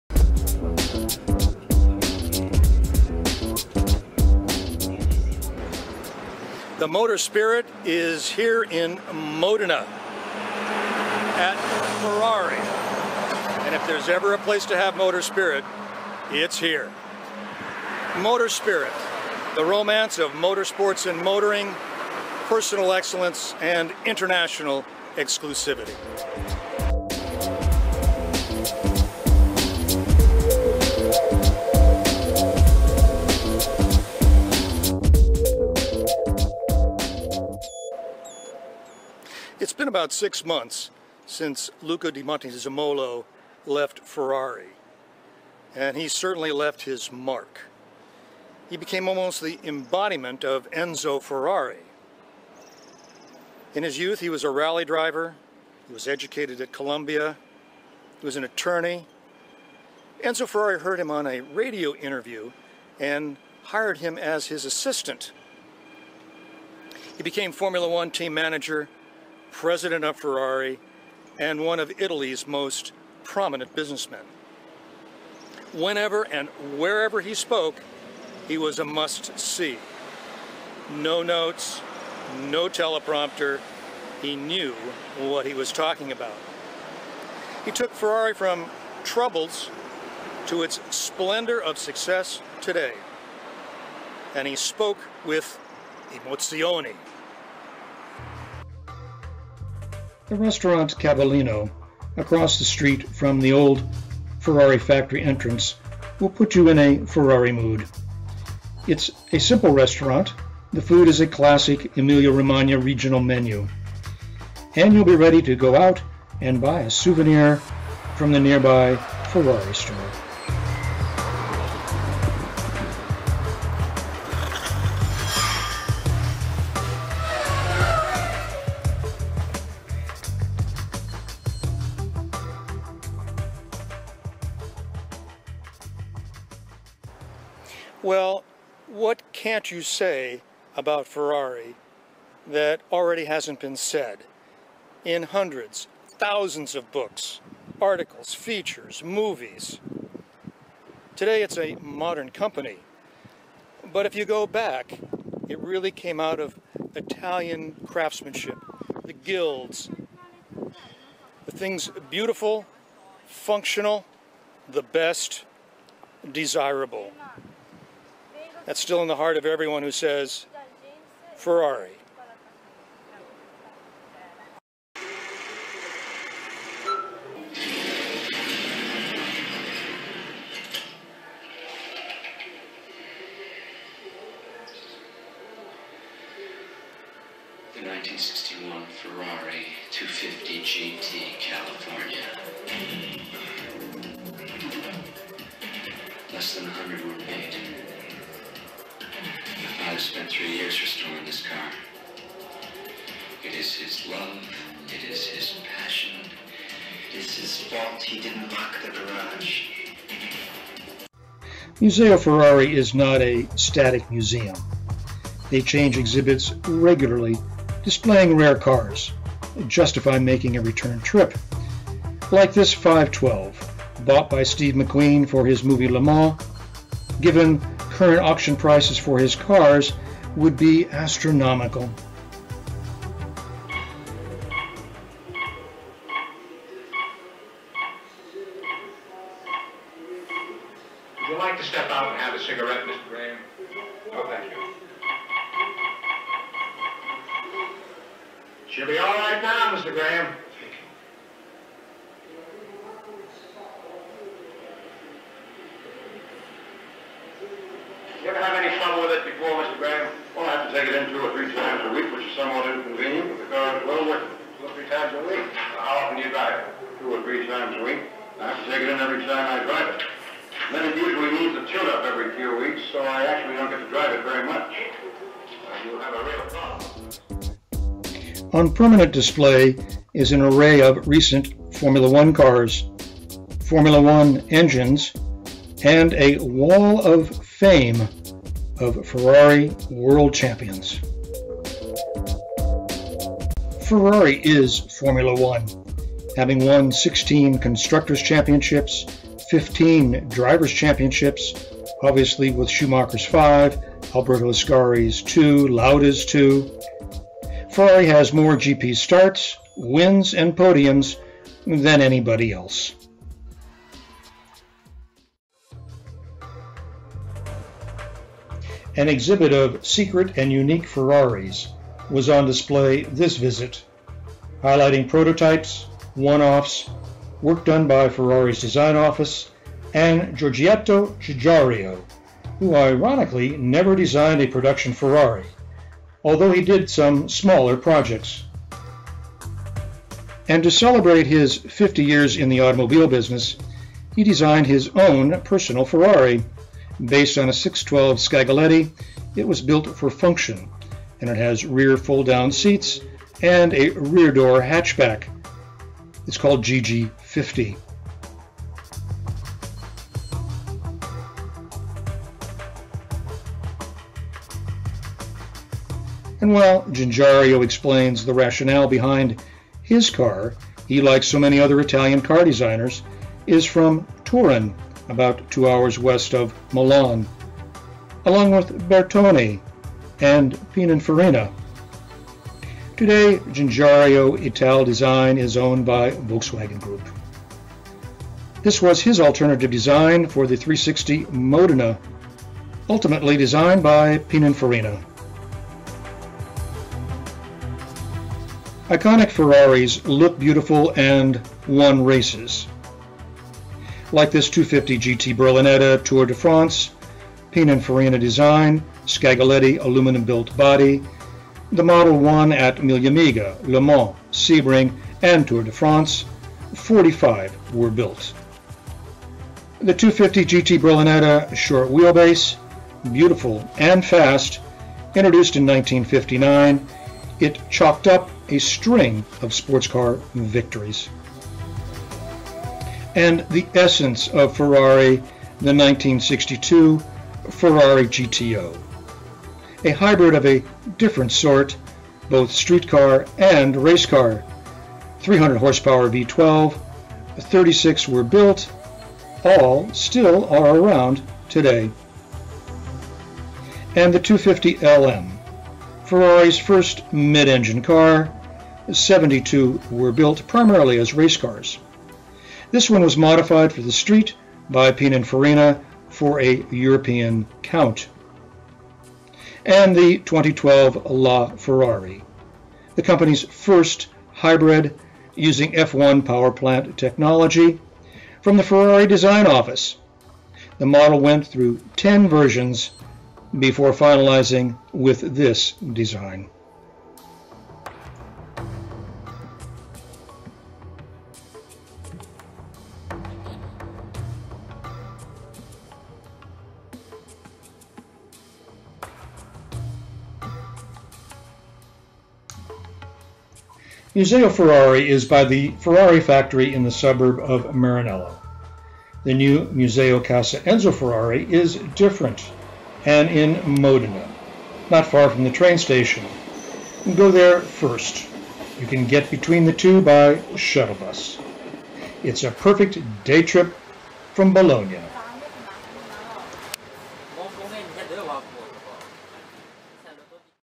themotorspirit.com: museo ferrari, may 2015.
motorspirit in modena at museo ferrari; lunch at ristorante cavallino.  on exhibit "secret and unique" ferraris. may 2015.
motorspirit is the romance of motorsports and motoring.
do you have the motorspirit.